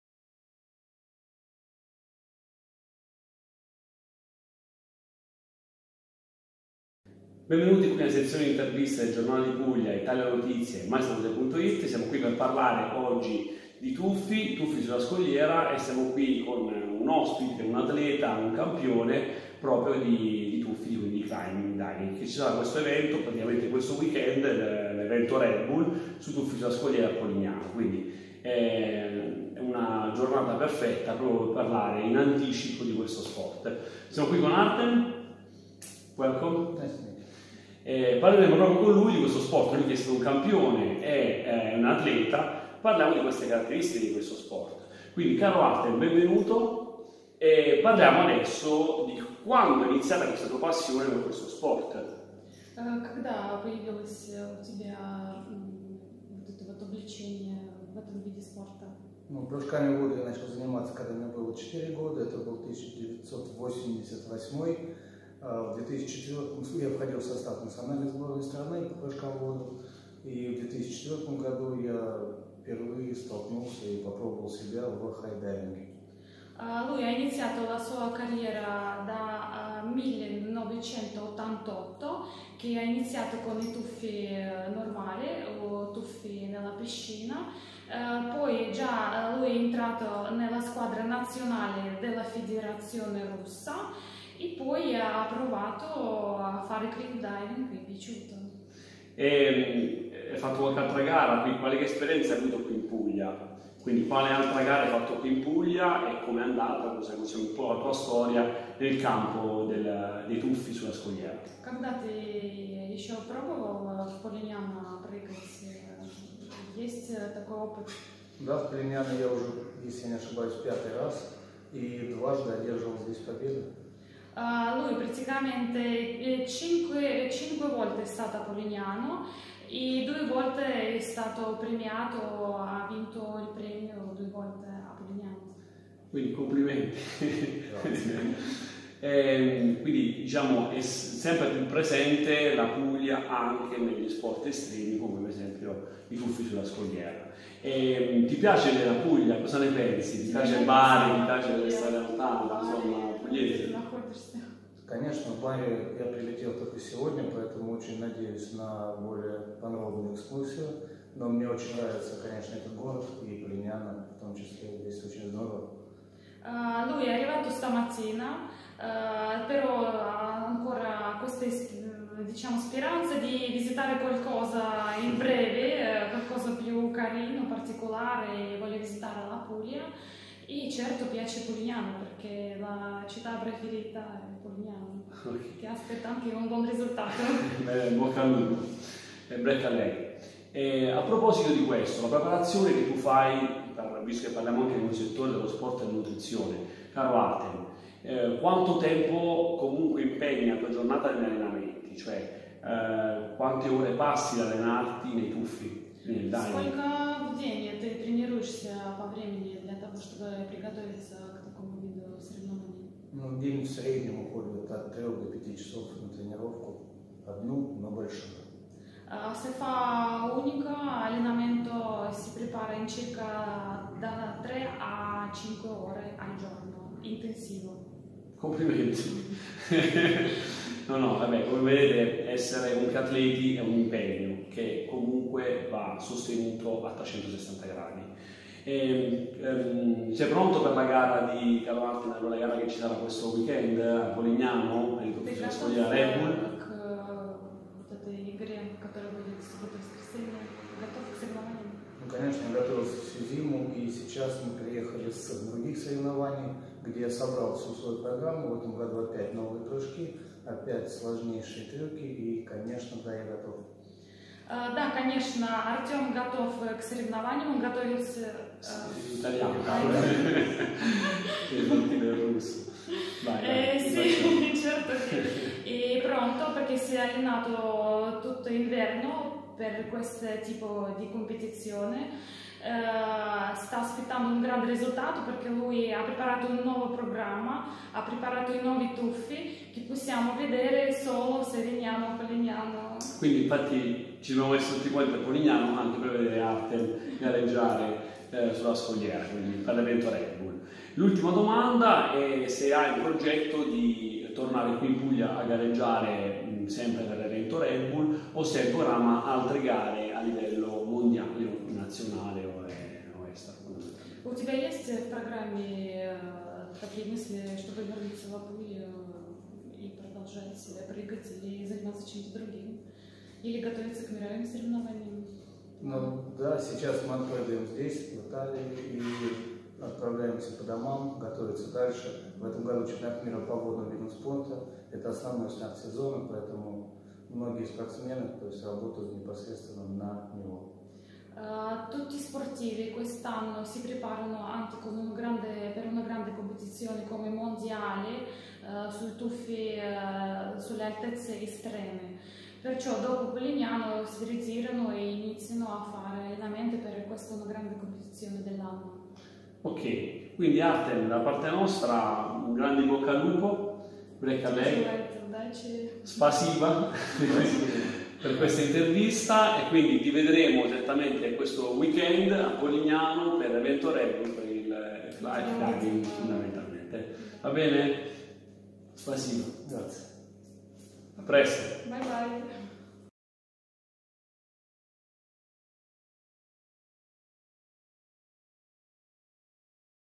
Red Bull Cliff Diving World Series 2019
"Red Bull Cliff Diving World Series 2019" Polignano a Mare. Intervista ad Artem Silchenko, High Diver. Intervista a cura di Edoardo Giacovazzo. Montaggio video a cura di Daniele Martini.